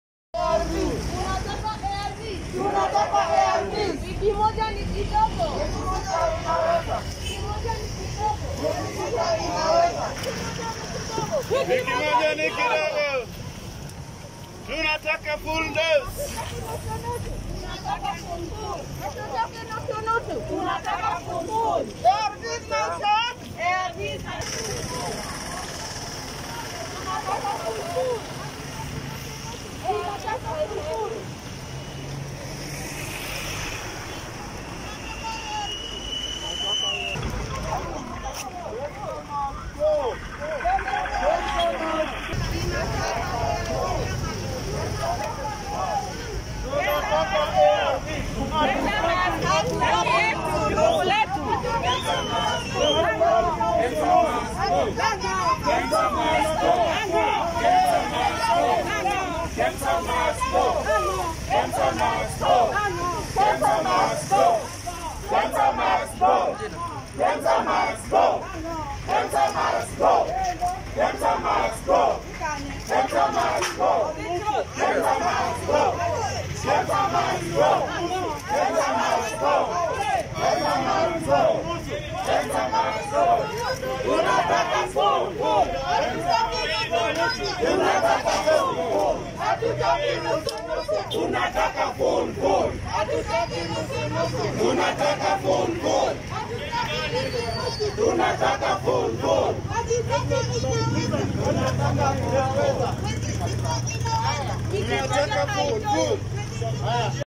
People living with HIV demonstrate in Kisumu over ARVs shortage
People living with HIV demonstrate in Kisumu over ARVs, want government to give them doses that will last six months.